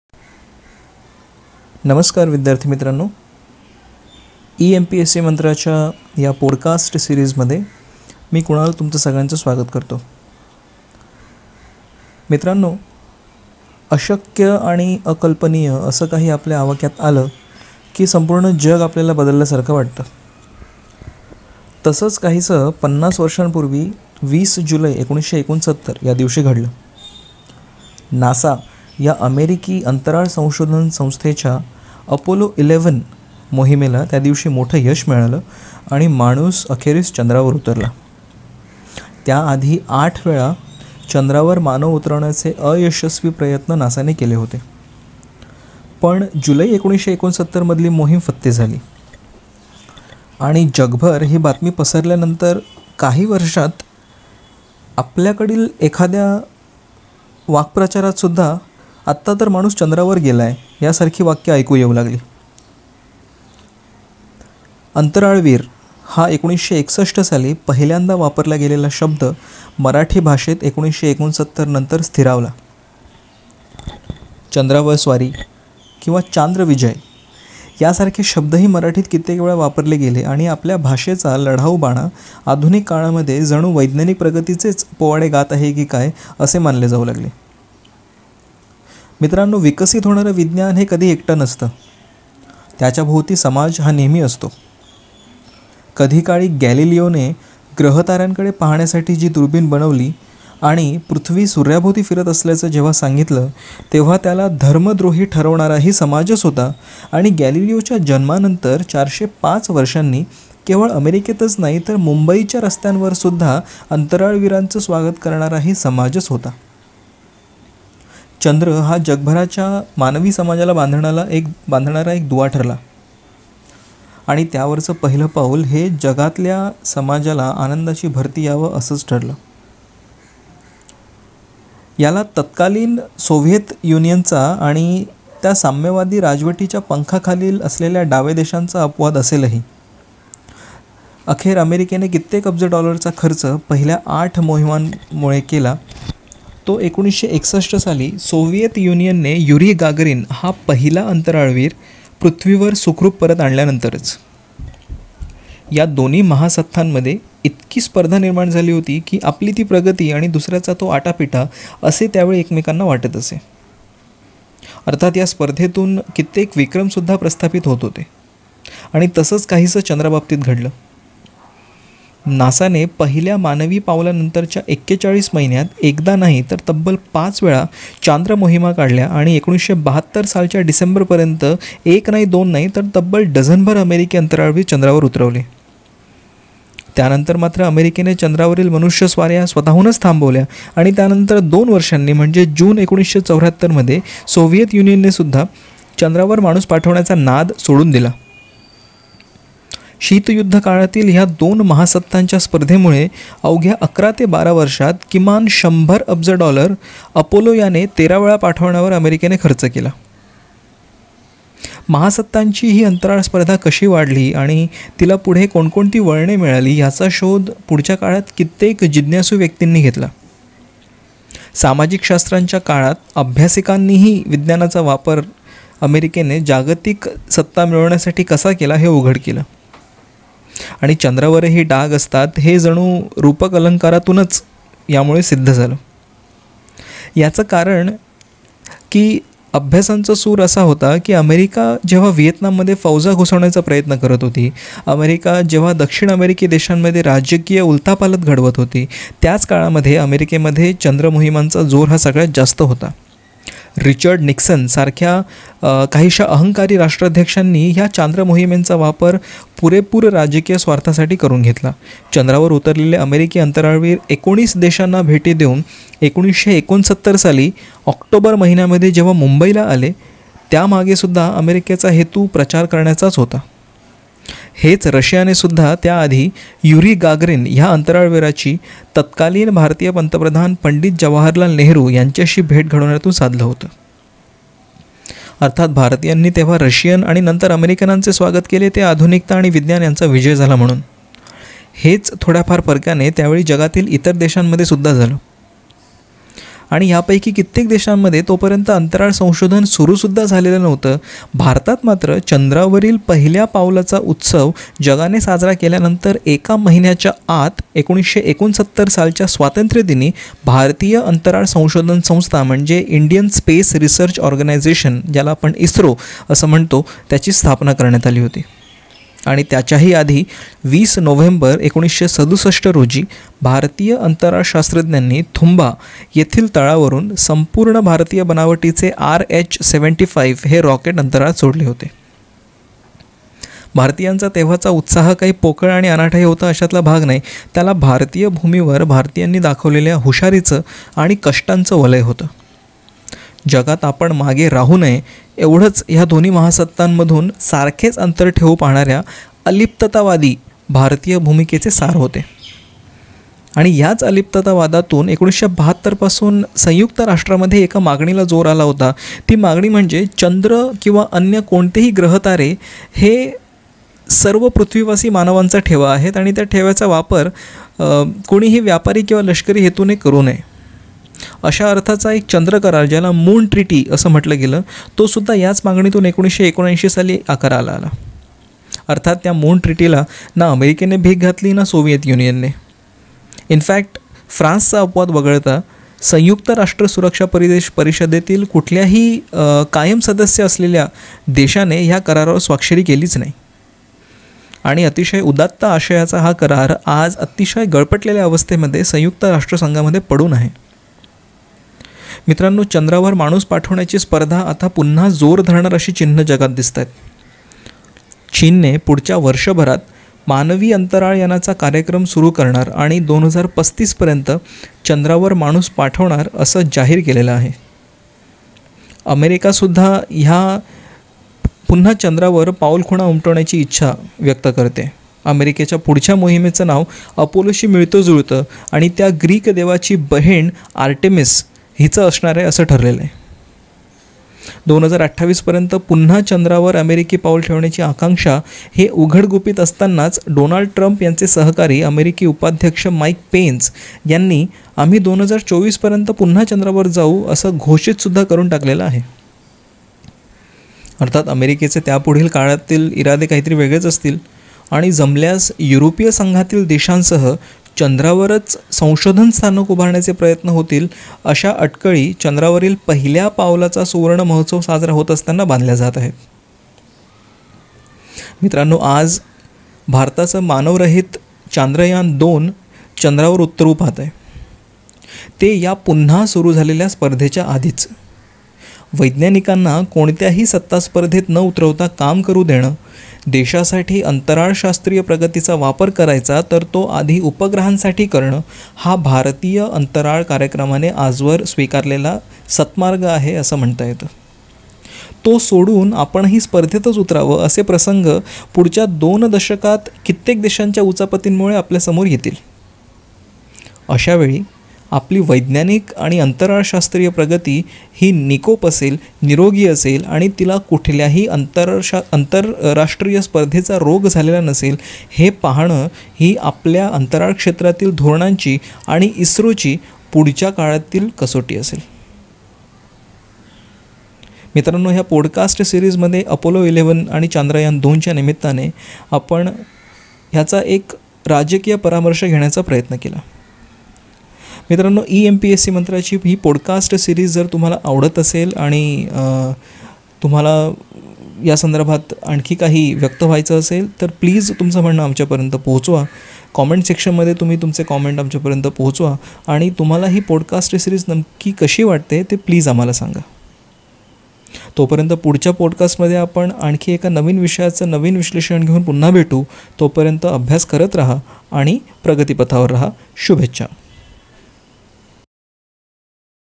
Podcast | अपोलो 11 आणि चांद्रयान-2 च्या निमित्ताने | eMPSC Mantra | Marathi
मित्रांनो, मानवाने चंद्रावर पाहिलं पाऊल ठेवणे आणि भारताच्या चांद्रयान मोहिमेचा दुसरा टप्पा यशस्वीपणे पार
पडणे या दोन वेगवेगळ्या कालखंडातील परंतु समान महत्त्व असलेल्या घटना. जग मानव चंद्रावर गेल्याचा सुवर्ण महोत्सव साजरा करत असताना भारताची ही अंतराळ झेप निश्चितच कौतुकास्पद आहे.